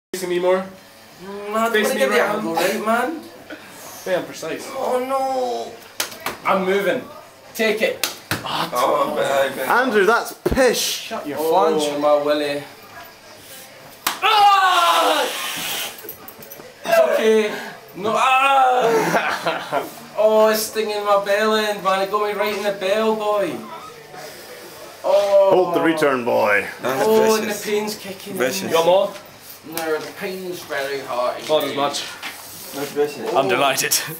ball in the balls
totally fucked 1 night and end up throwing a juggeling ball at eachother